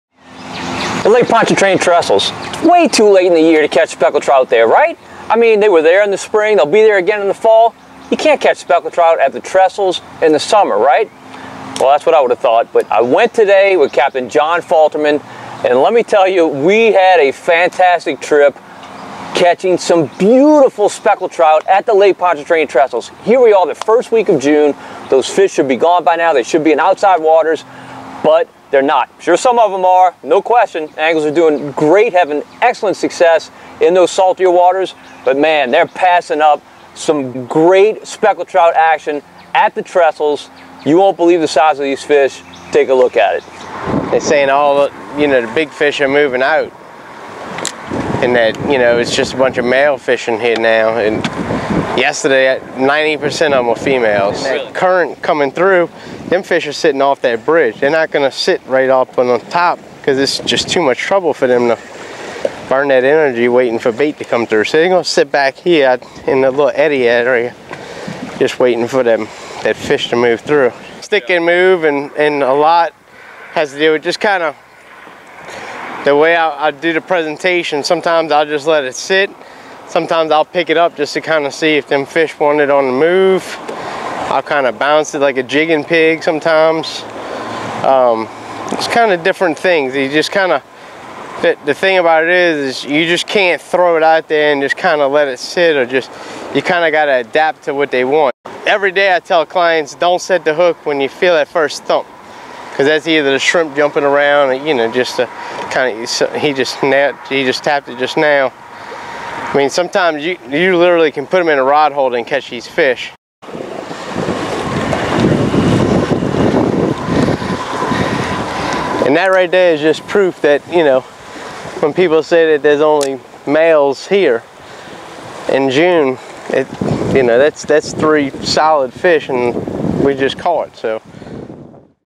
Summertime speckled trout at the Lake Pontchartrain Trestles
Even though South Louisiana is in meteorological summer, Capt. John Falterman shows there are still plenty of speckled trout at the Lake Pontchartrain Trestles, and they should be there for a while.